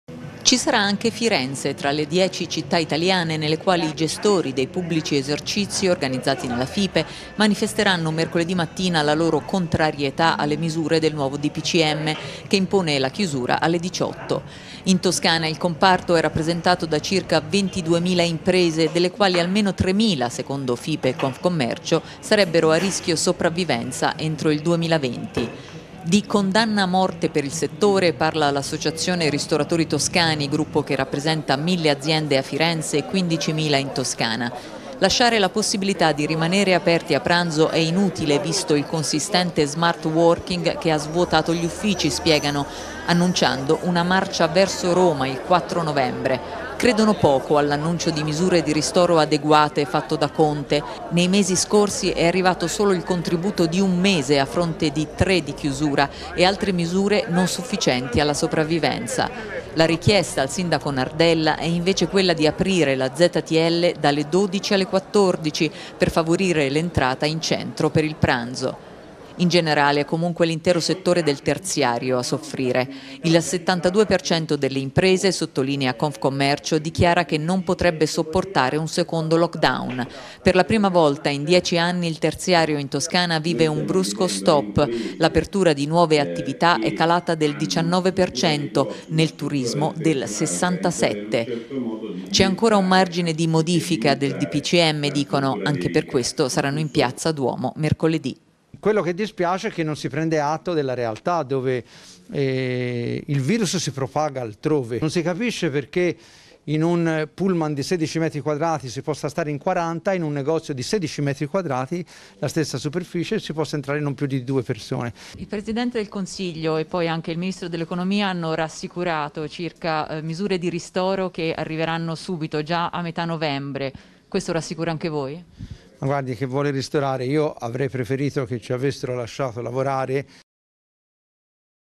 2020-10-26 FIRENZE - RISTORATORI IN PIAZZA MERCOLEDI' CONTRO DPCM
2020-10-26 FIRENZE - RISTORATORI IN PIAZZA MERCOLEDI' CONTRO DPCM